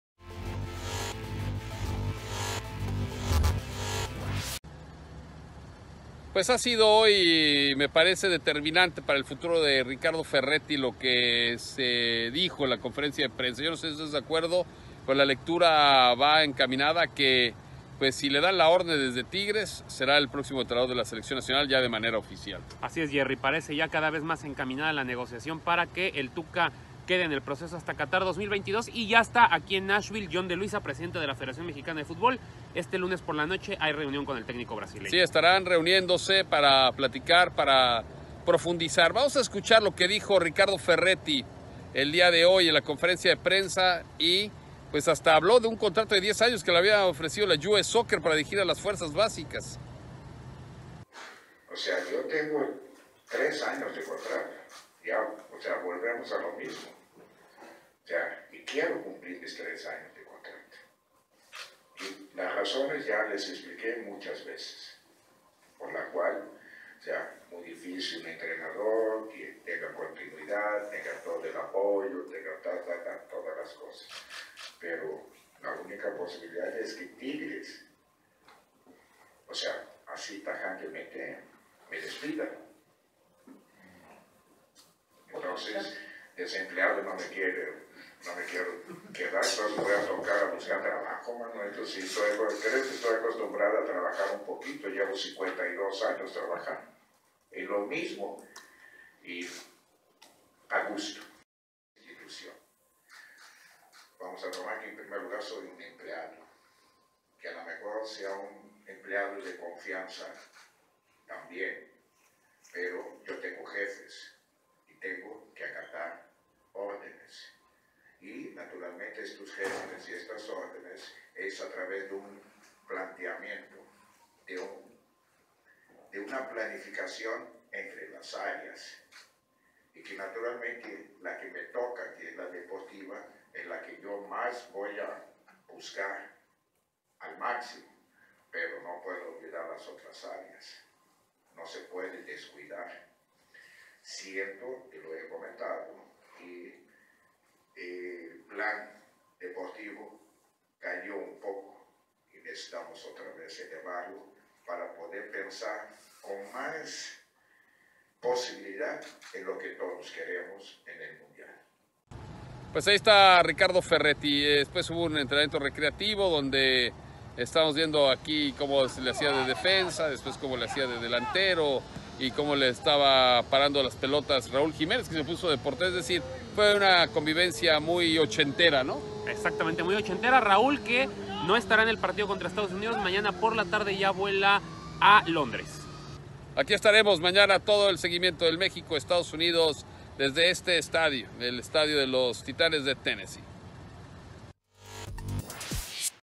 Lista la Selección Mexicana para enfrentar a Estados Unidos
Habló Ferretti en conferencia de prensa y entrenó el Tricolor de cara al partido frente a Estados Unidos, el reporte nos lo traen Gerardo Velázquez de León y Daniel Blumrosen